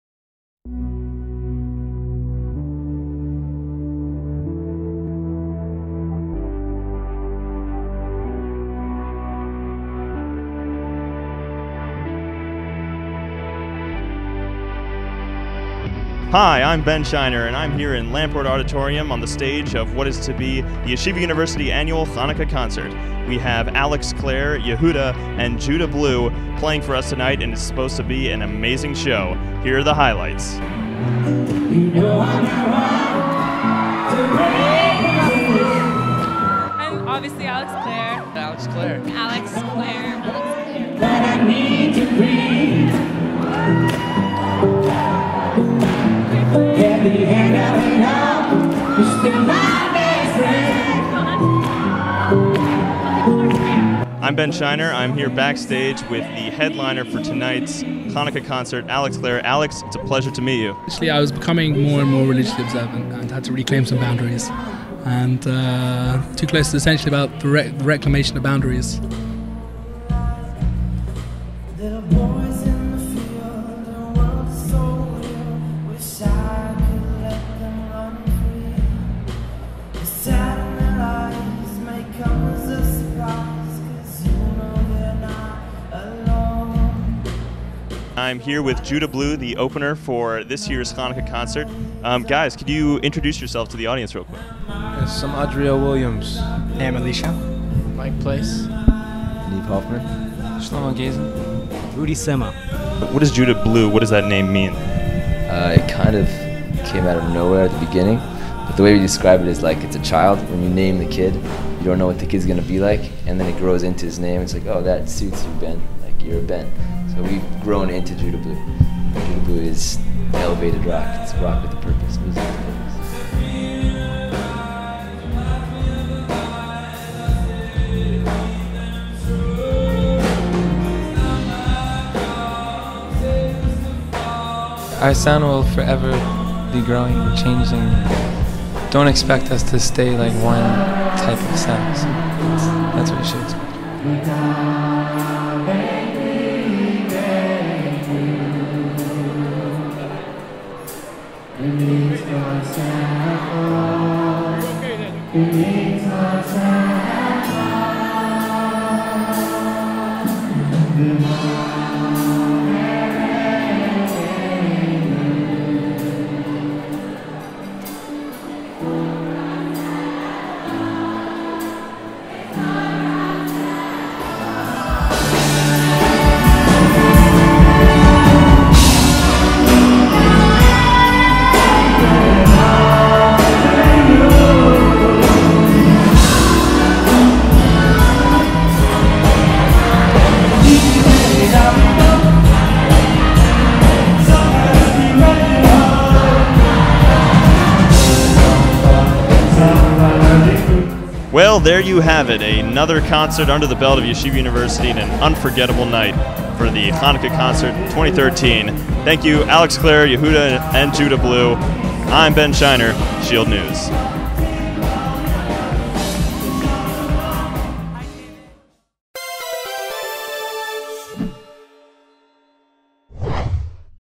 Shield News: Chanukah Concert recap
Check out this recap video from the Yeshiva University undergraduate student broadcast channel of the YU annual chanukah concert.  The concert featured Alex clare and Yehuda! and Judablue.  Watch to see if you got on video!